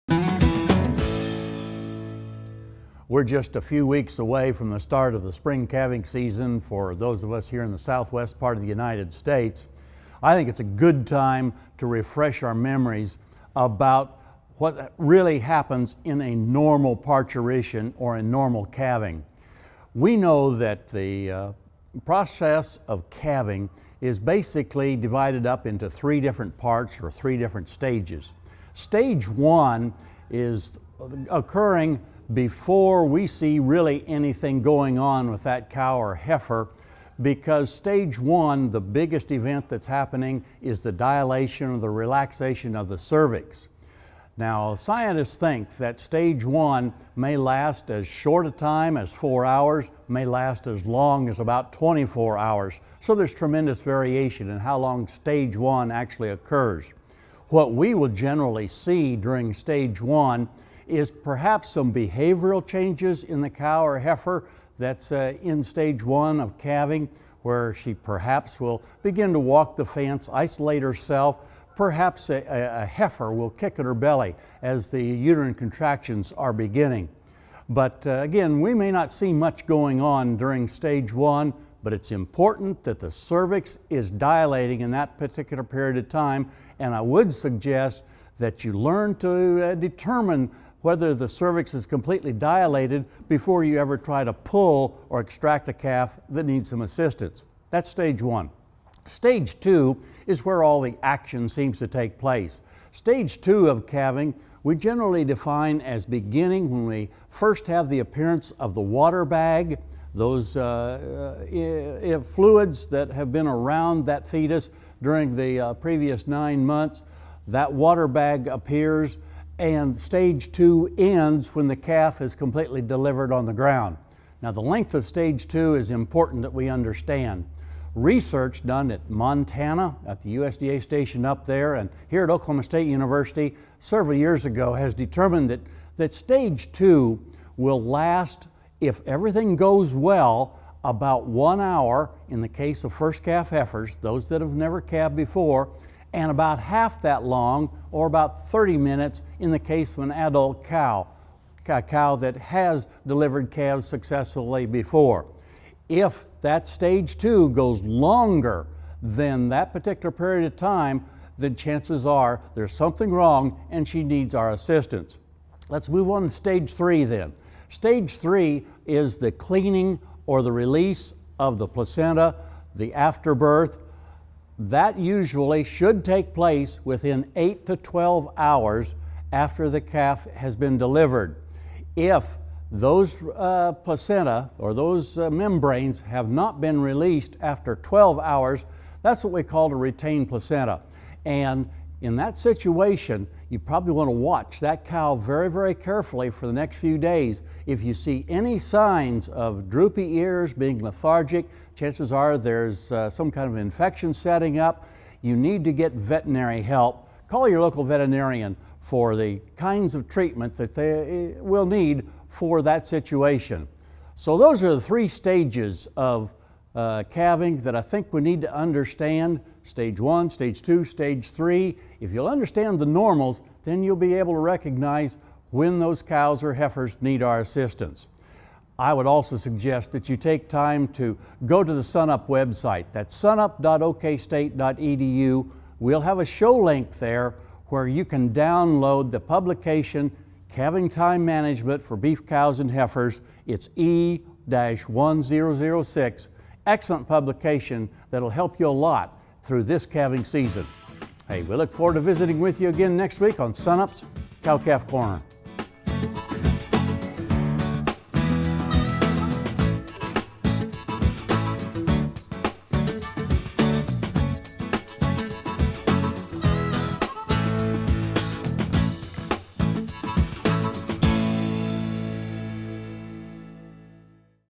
Cow-Calf Corner - The stages of calving (1/12/19)
Glenn Selk has advice for the 3 stages of calving.